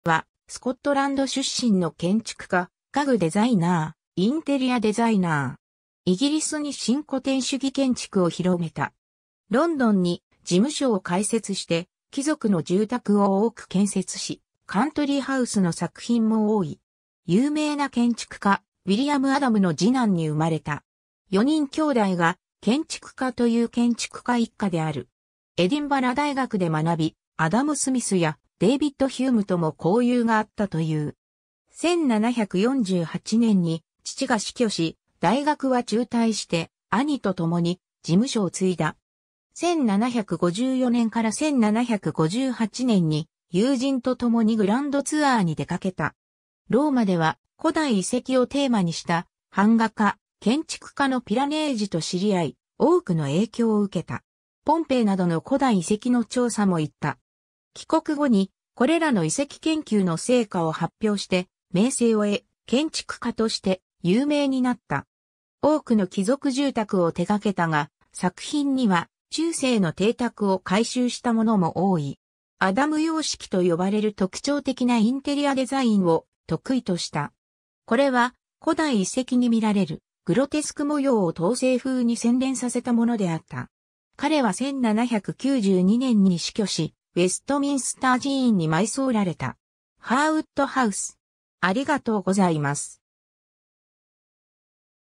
ロバート・アダム
ロバート・アダム ロバート・アダム（Robert Adam, 1728年7月3日 - 1792年3月3日）は、スコットランド出身の建築家、家具デザイナー、インテリアデザイナー。
イギリスに新古典主義建築を広めた。
ロンドンに事務所を開設して、貴族の住宅を多く建設し、カントリー・ハウス（貴族が荘園に建てた邸宅）の作品も多い。
 有名な建築家ウィリアム・アダムの次男に生れた。
4人兄弟が建築家という建築家一家である。
エディンバラ大学で学び、アダム・スミスやデイヴィッド・ヒュームとも交友があったという。
1748年に父が死去し、大学は中退して兄と共に事務所を継いだ。
1754年から1758年に友人とともにグランドツアーに出かけた。
ローマでは古代遺跡をテーマにした版画家・建築家のピラネージ（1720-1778年）と知り合い、多くの影響を受けた。
ポンペイなどの古代遺跡の調査も行った。
帰国後にこれらの遺跡研究の成果を発表して名声を得、建築家として有名になった。
 多くの貴族住宅を手がけたが、作品には中世の邸宅を改修したものも多い。
アダム様式 (Adam style) と呼ばれる特徴的なインテリアデザインを得意とした。
これは古代遺跡に見られるグロテスク模様を当世風に洗練させたものであった。
 彼は1792年に死去し、ウェストミンスター寺院に埋葬られた。
 ハーウッド・ハウス